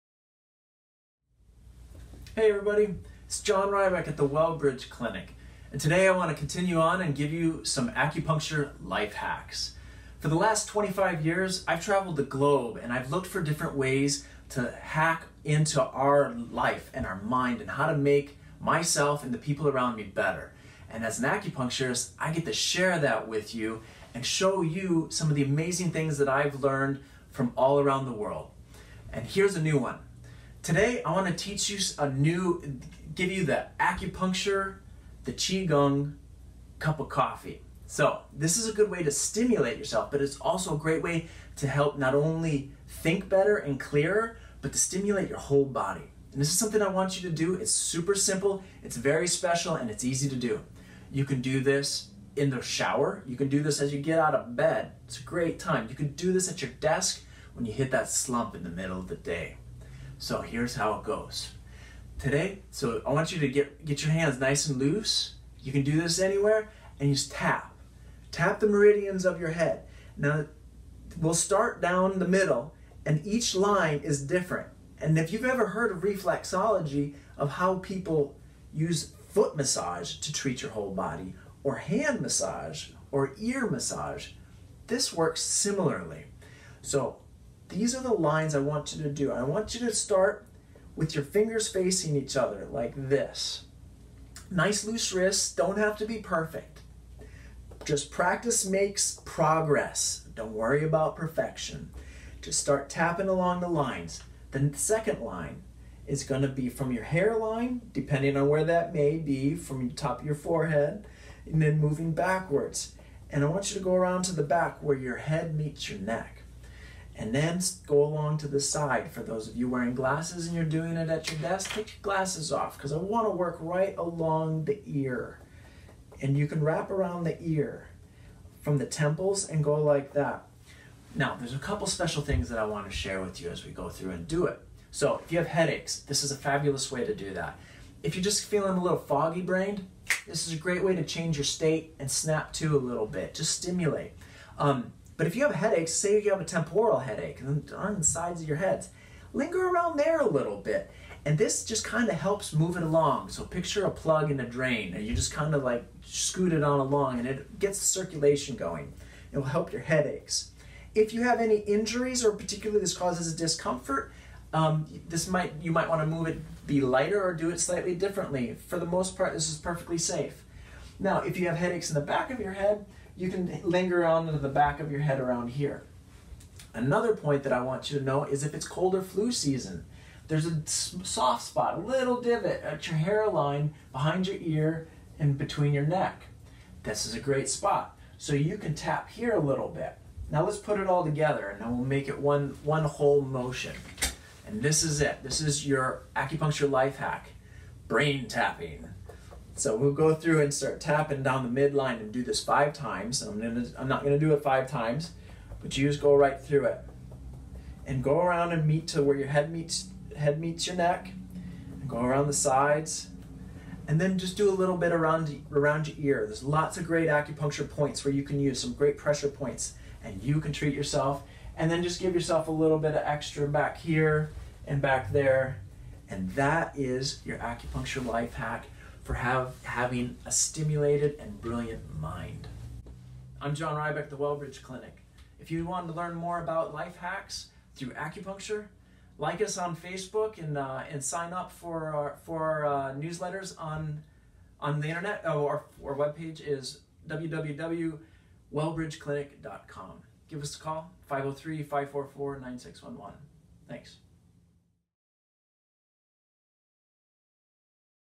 BIO HACK: Waking Up Your Brain and Reducing Headaches!!!
Need to wake your brain up?  Relax a little?  Headaches bothering you? You need to watch this bio hack that will change your life!  Try it out and see a difference in your health.

Some people call it Qigong...some people call it mechanotransduction.  What ever word you like...it means you are taking Kinetic Energy (tapping on your head) and creating Biochemical Energy (producing chemicals in your brain and body).

It is also like a self massage or acupressure/reflexology.  Our head is wrapped in fascia and muscles that can cause tension headaches amongst other problems.  Tapping helps relax the muscles.

Check us out at The WellBridge Clinic.  Your success is our purpose. Expect more from your health care.